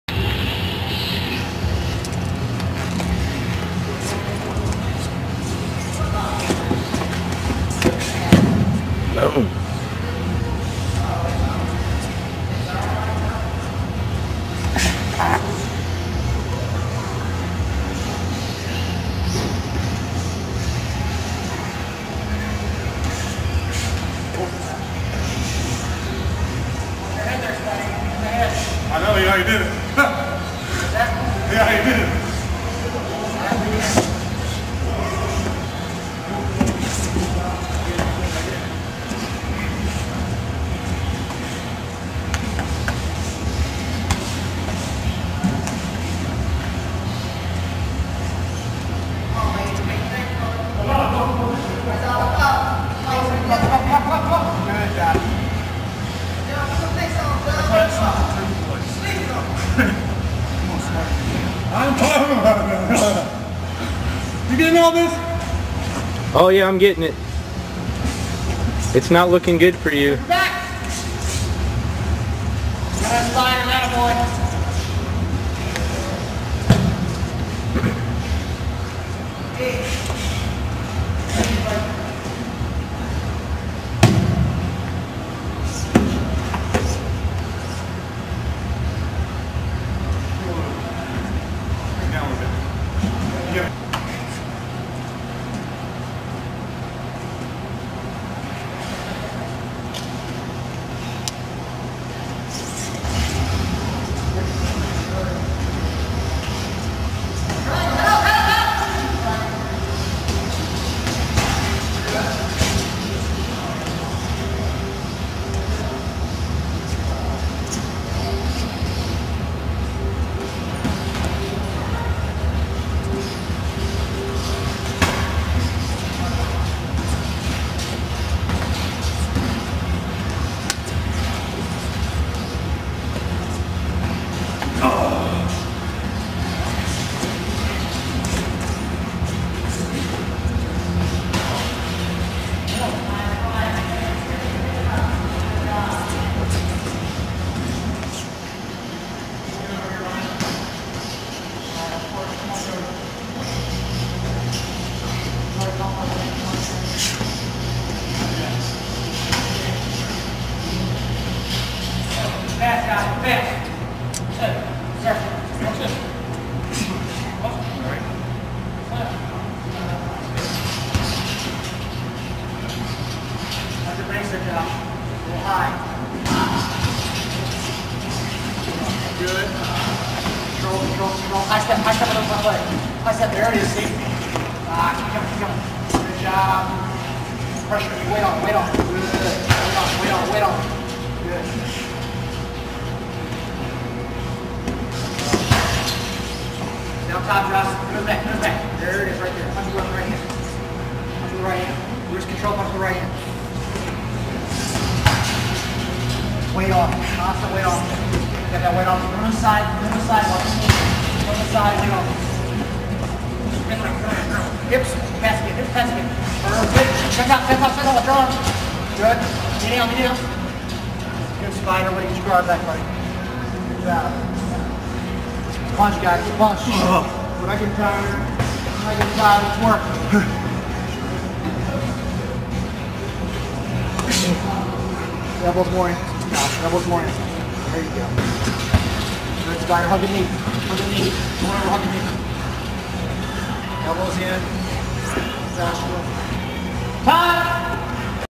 Josh Schockman vs Sean 'spyder' Pulizzano
Some MMA sparring in the combat athlete class at Nashville MMA a couple years back.